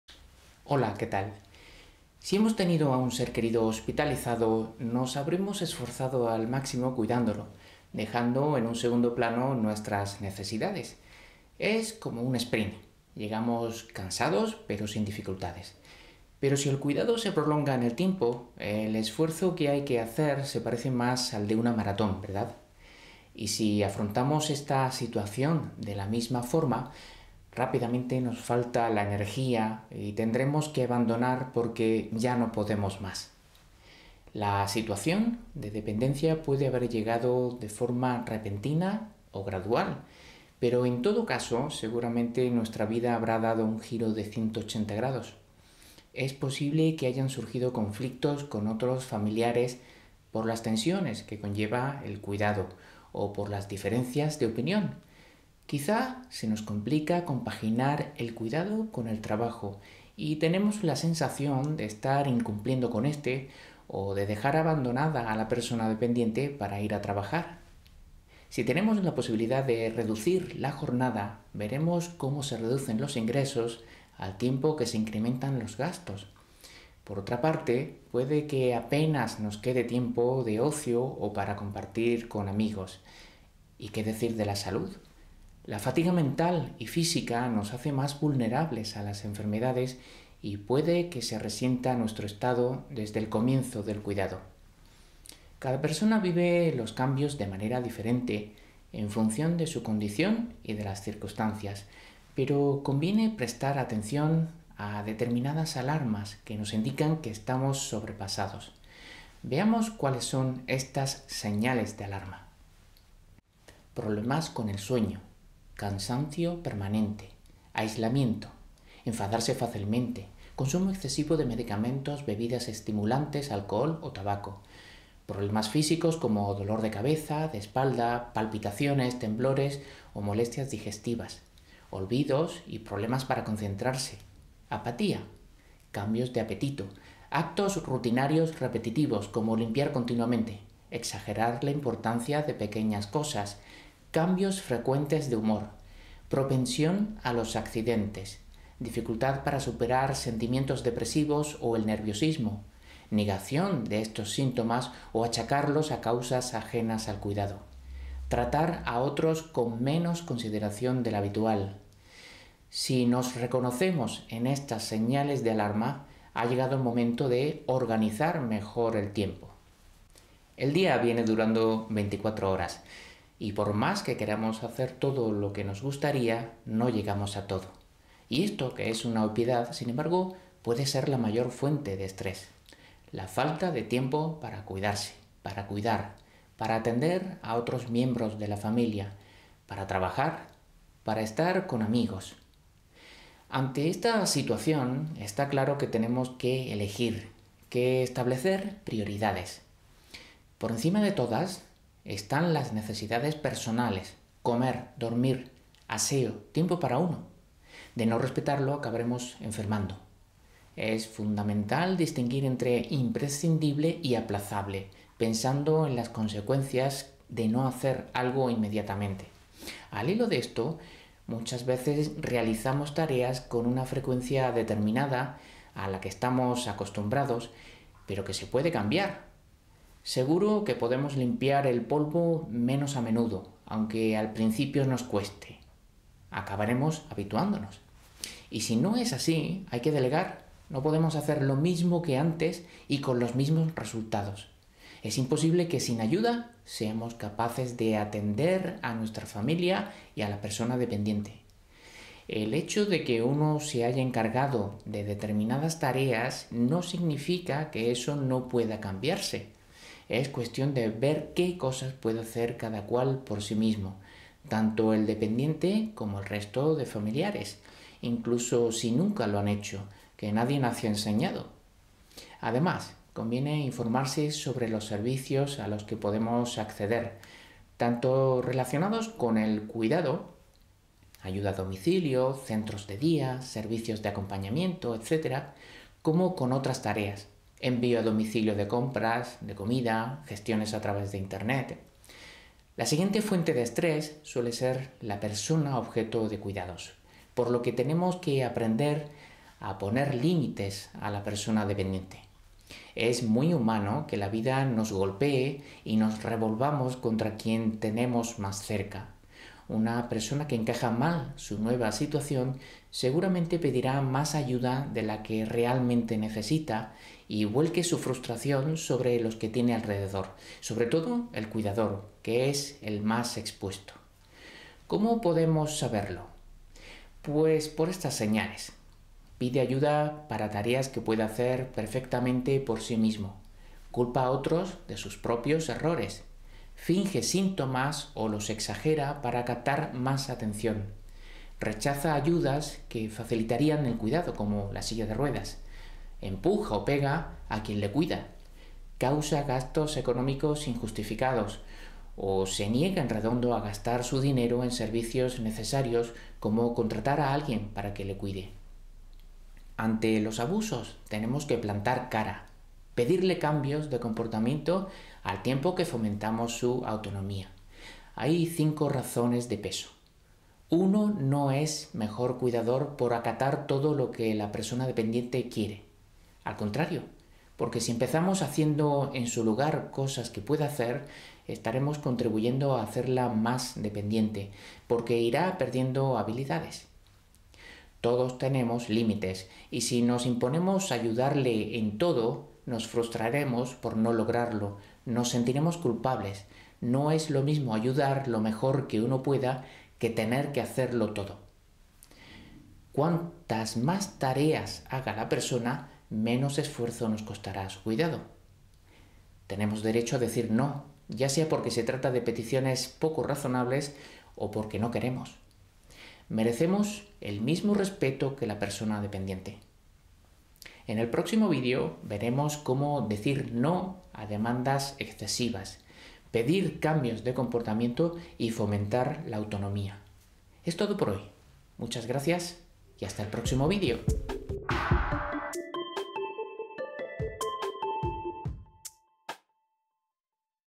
Aux soins des personnes à charge, I (sous-titres en français)
Premier volet de cette série consacrée au soutien aux aidants naturels. Nous commençons par des stratégies d'autosoins: signes avant-coureurs, comment mieux s'organiser et pourquoi imposer des limites à la personne dépendante.